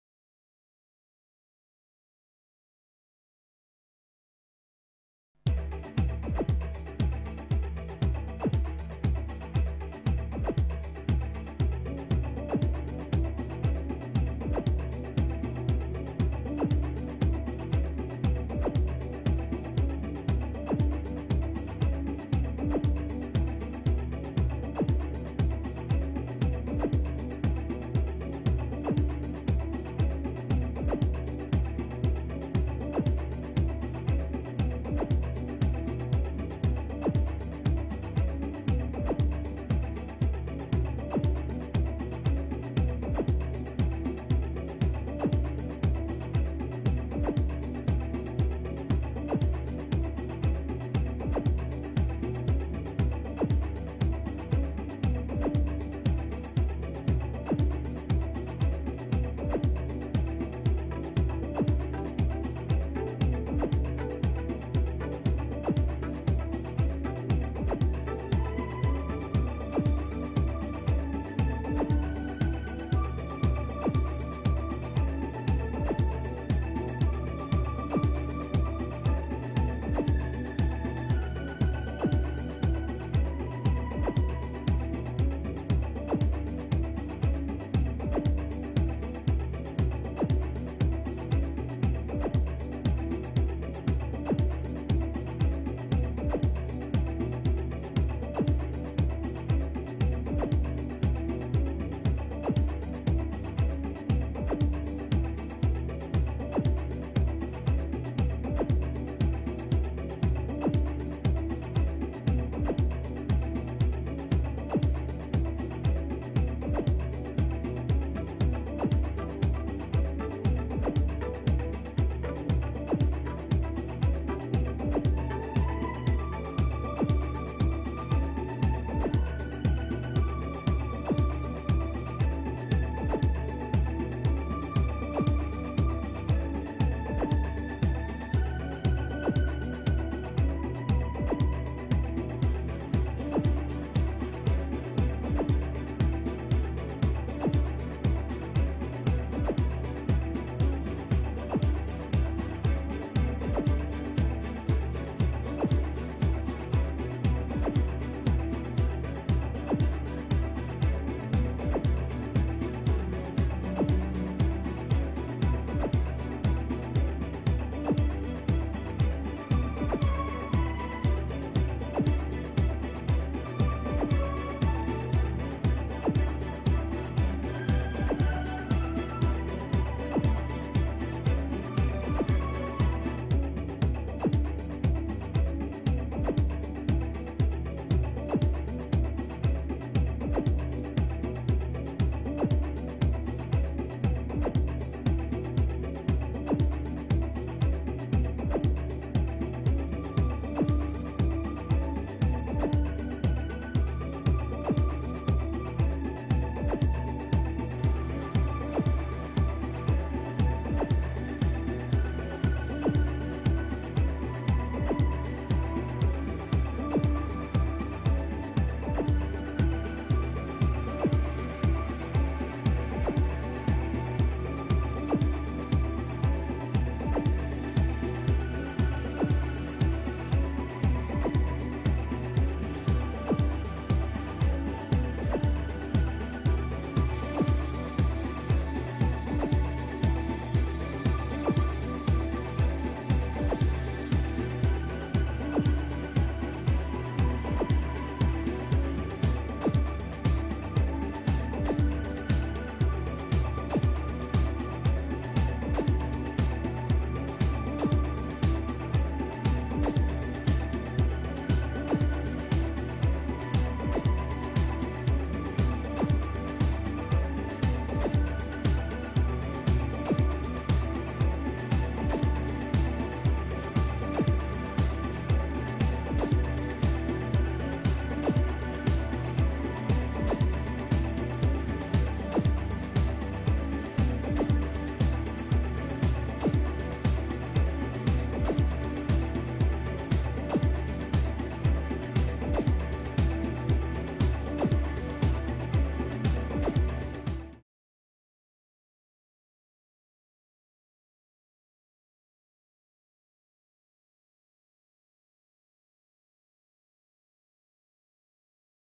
Glasgow City of Light 2003 LUCI
Early lighting strategy film for the city of Glasgow.